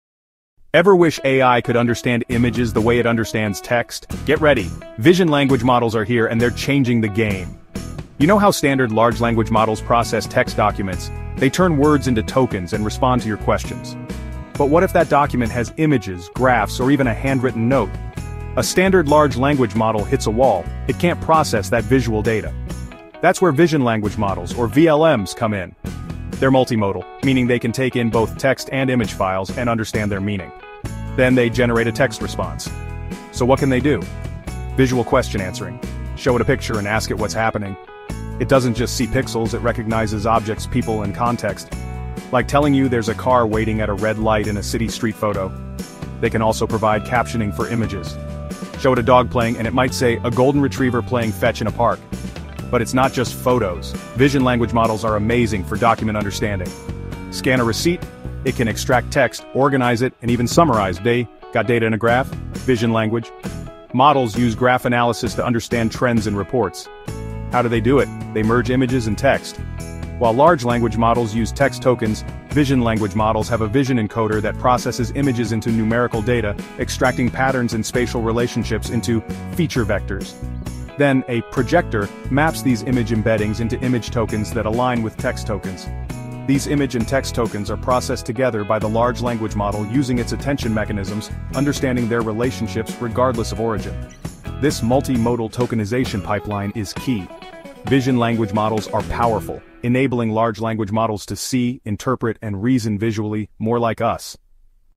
Vision Language Models: Al's New Superpower!
Unlock the future of AI with Vision Language Models (VLMs)! In this exciting YouTube Shorts, we delve into how VLMs revolutionize the way artificial intelligence interprets images, graphs, and text simultaneously. Say goodbye to the limitations of traditional Large Language Models (LLMs) and discover how VLMs excel in tasks like Visual Question Answering and image captioning. Learn how these advanced models merge visual data with textual information, allowing AI to interpret, reason, and respond just like us!